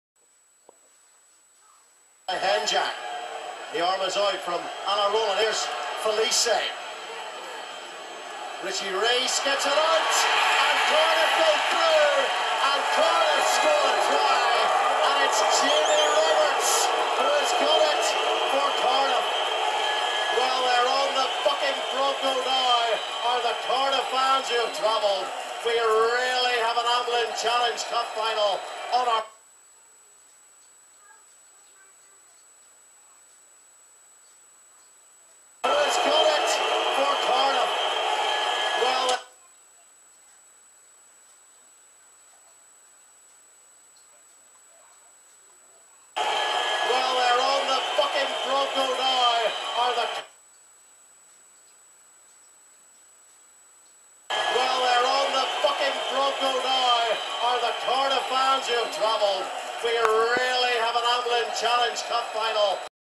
Cardiff v Toulon Jamie Roberts Try but what did he say ????
Now we all get excited when we see a great try but what on earth did he say when he was running back. I think or perhaps hope he was on about something about Rodeo riding.